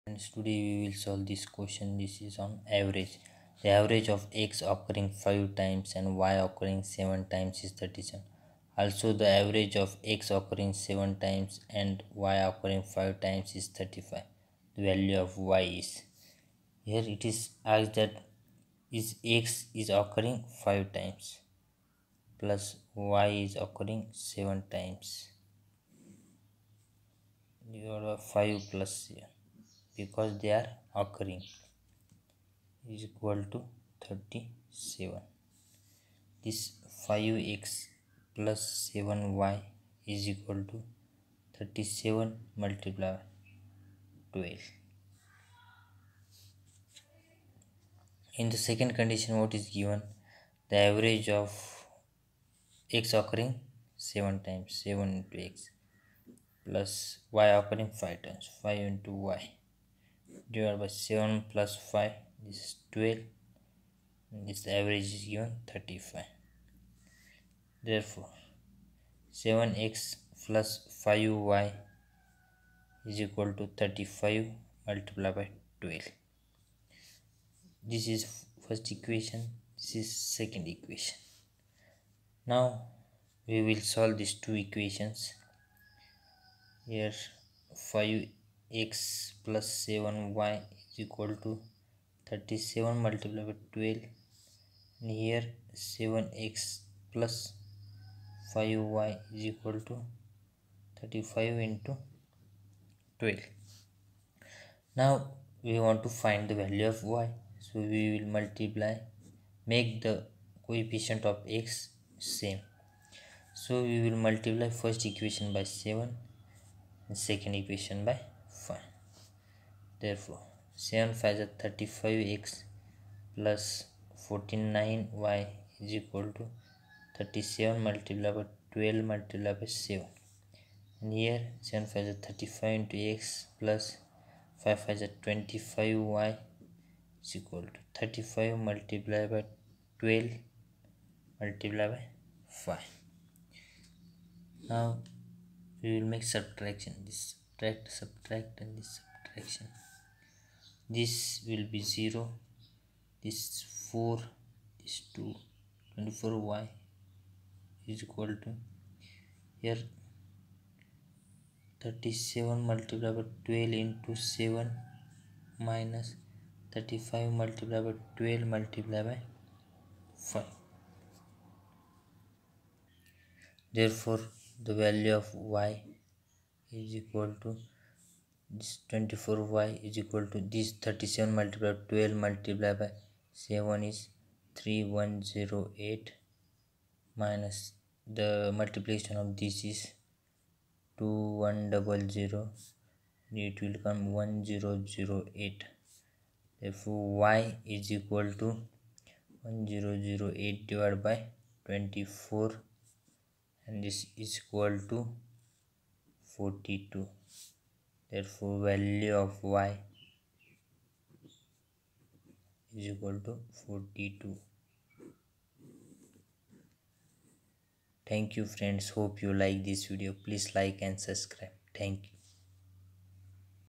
The average of X occuring 5times,y occuring 7 times is 37.avg of X occuring 7times and y occurs 5...
SSC CGL2020